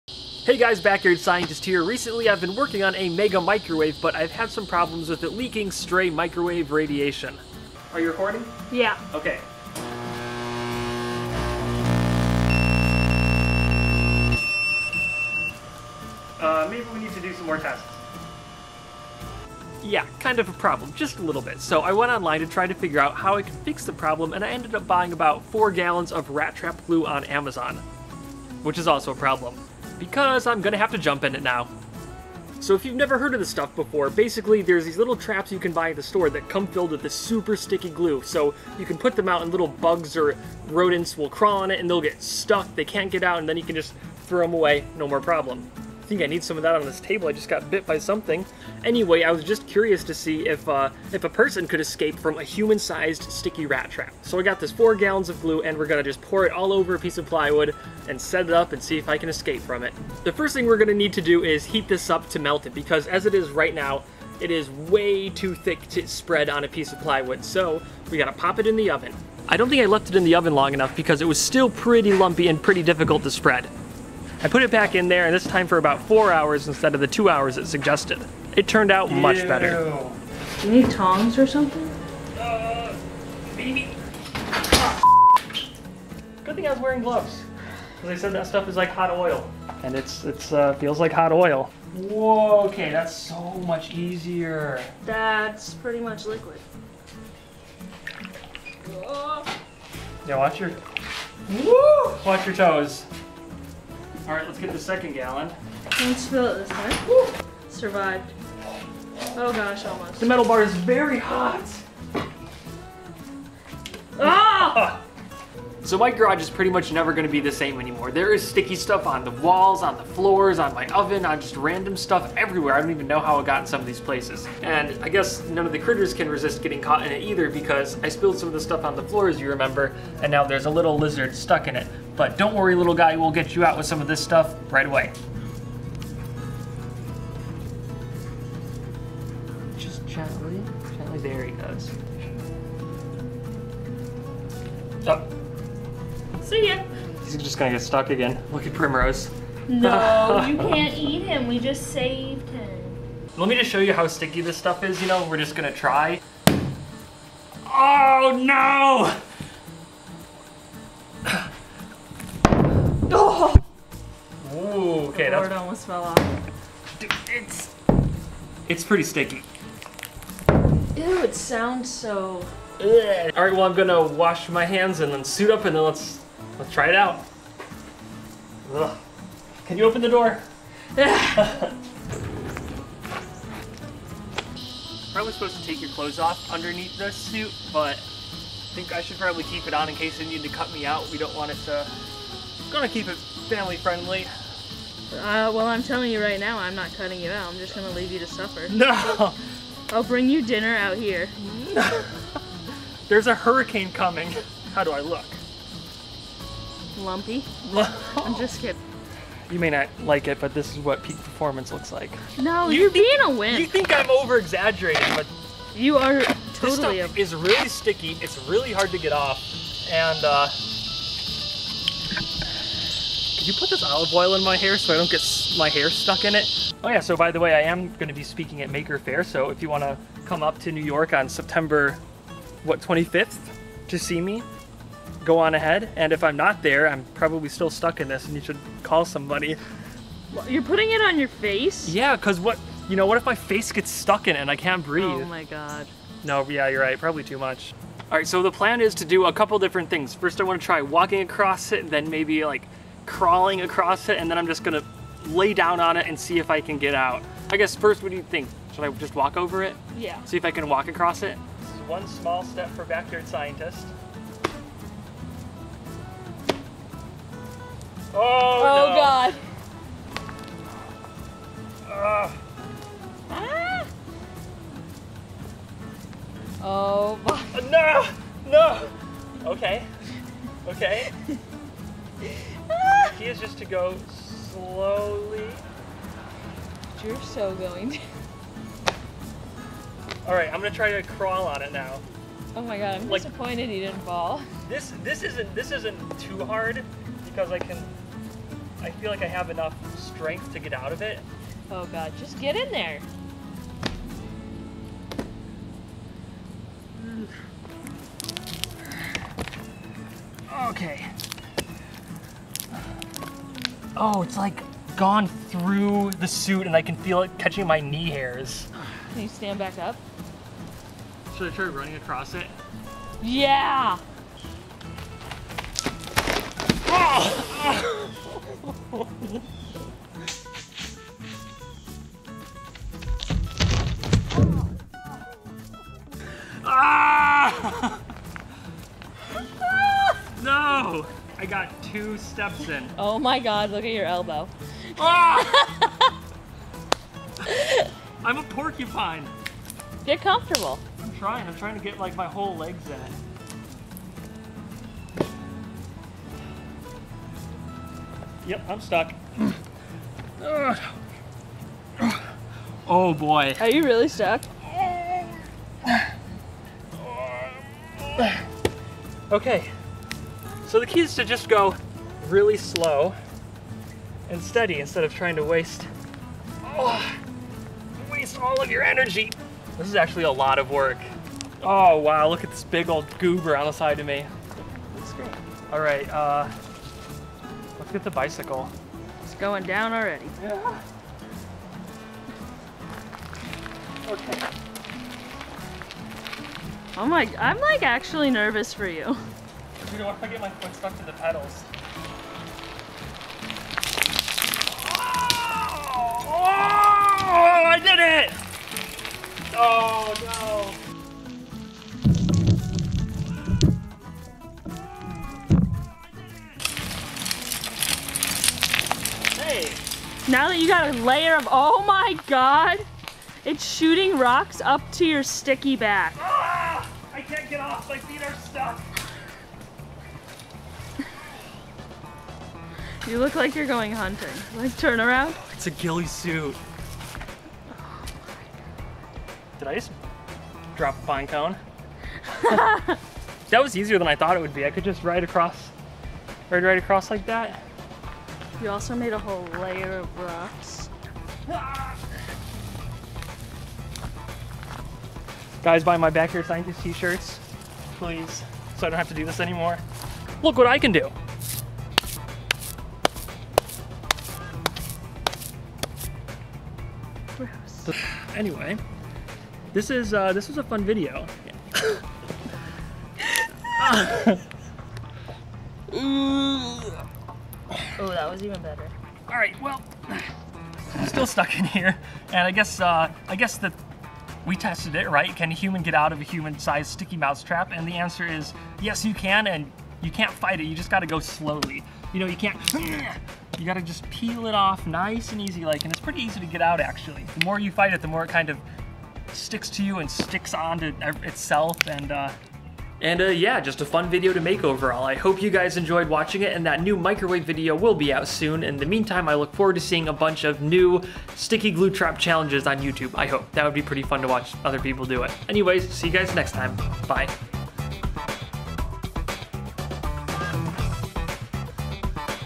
Can a Person escape a human-sized Glue Trap?
After struggling with my mega-microwave I decided to have some fun.     I got 4 gallons of bulk glue for mouse traps because I was curious if a person could escape from a human-sized glue trap.
I never saw anybody try it before so I was curious. At first I thought about getting a few hundred of the regular sized glue traps from the store and stapling them to the plywood but decided the bulk glue would be more economical and cover more surface area. It was not a cool color like the stuff in the store but I don't mind.